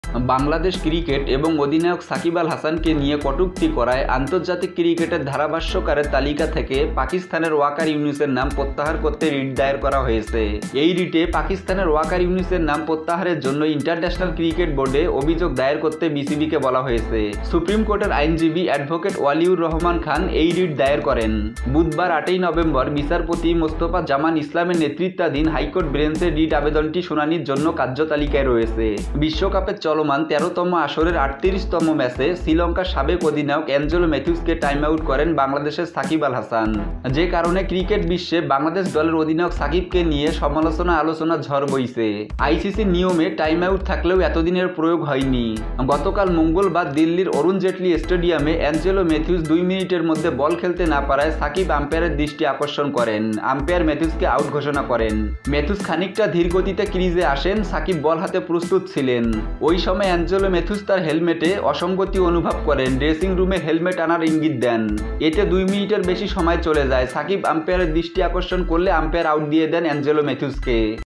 সাকিবের পক্ষে উচ্চ আদালতে বিসিবি ও আইনজীবীরা | Shakib Al Hasan | Waqar Younis | Cricket News
Job circular everyday on social media links 🔻